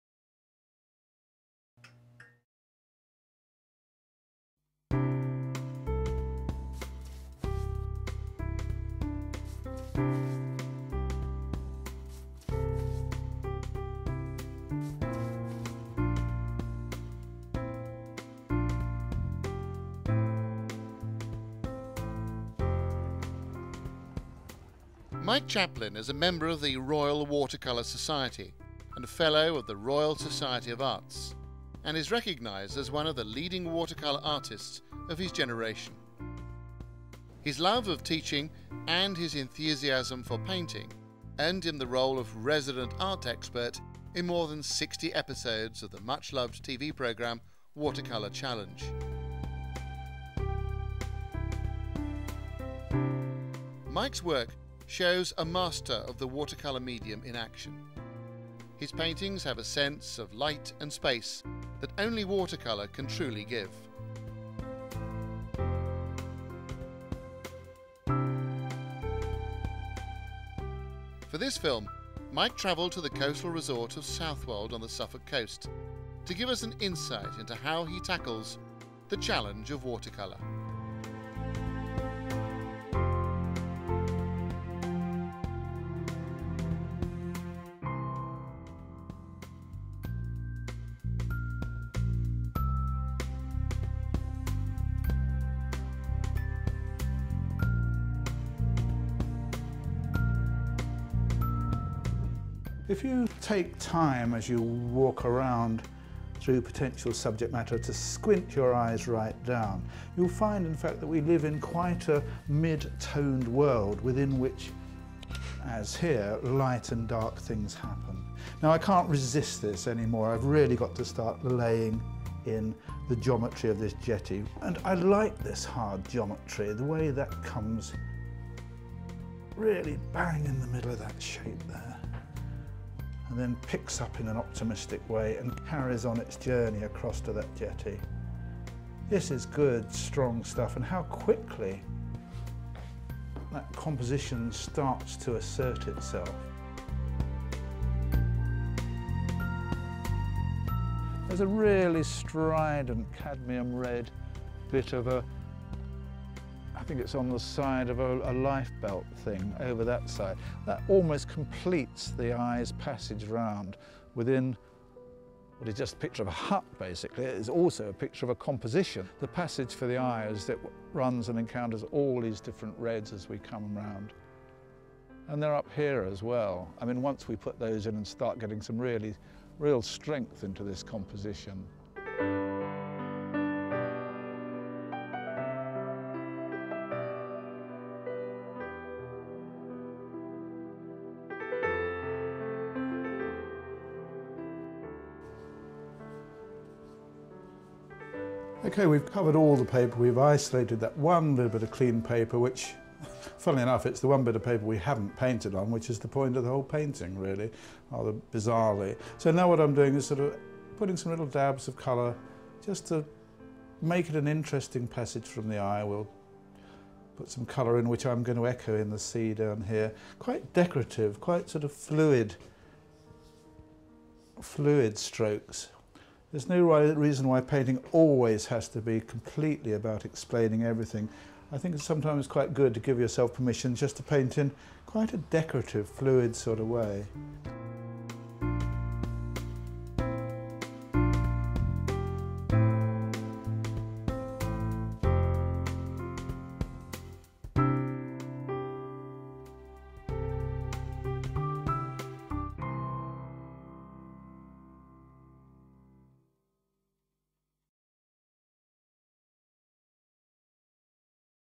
The Challenge of Watercolour with Mike Chaplin RWS
Mike Chaplin is a member of the Royal Watercolour Society and a Fellow of the Royal Society of Arts and is recognised as one of the leading watercolour artists of his generation. His love of teaching and his enthusiasm for his paining was evident in his celebrated role as resident art expert in more than 60 episodes of the much loved T.V. programme Watercolour Challenge.

Mike's work shows a master of the watercolour medium in action. His paintings have a sense of light and space that only a watercolour can give. For this film Mike traveled to the coastal resort of Southwold on the Suffolk coast to give us an insight into how he tackles the challenge of watercolour.